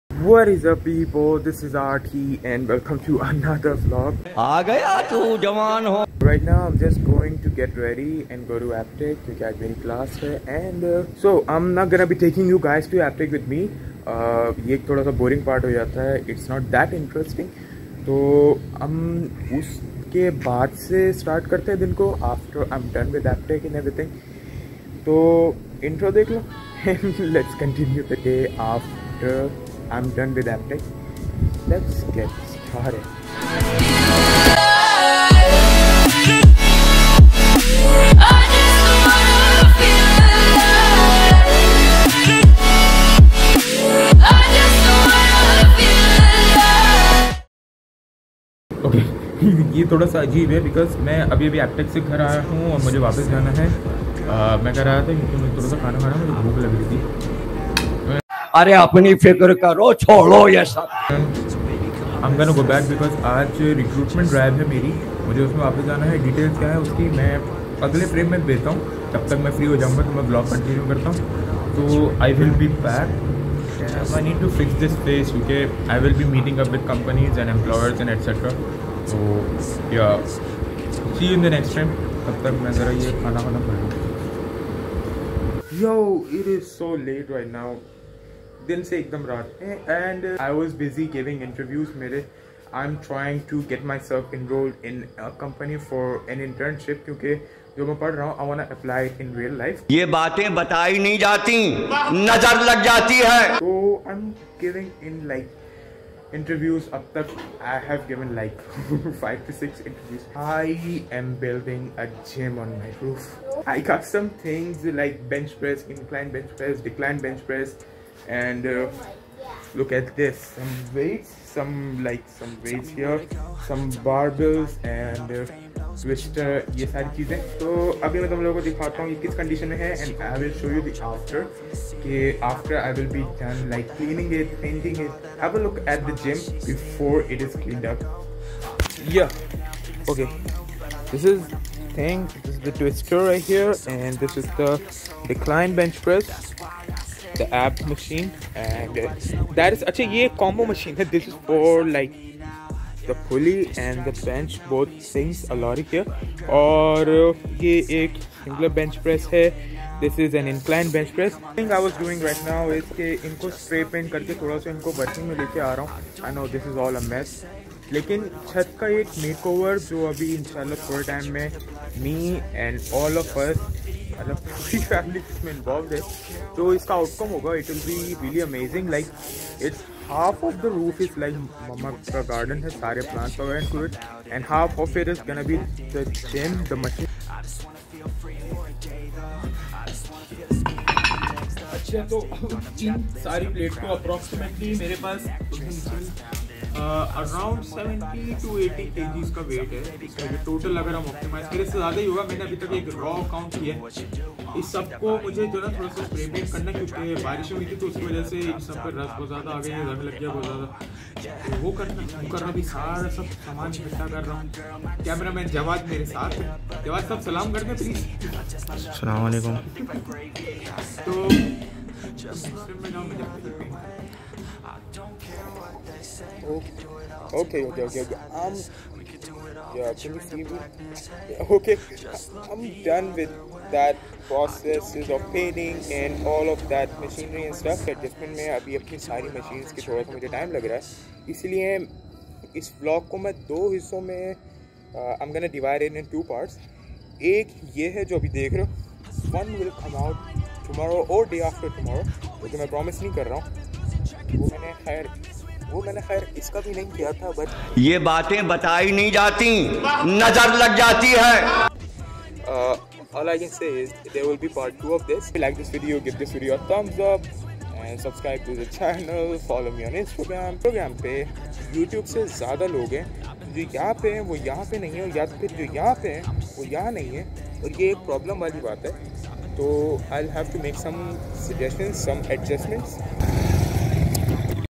BUILDING A GYM AT HOME ft. Syed Ghulam-e-Abbas Jafri ( LADDAN JAFRI ) - Vlog
THIS IS JUST FOR LIGHT HEARTED ENTERTAINMENT, May Allah Make ways Of Akhirah easy for Mr. Syed Ghulam-e-Abbas Jafri ..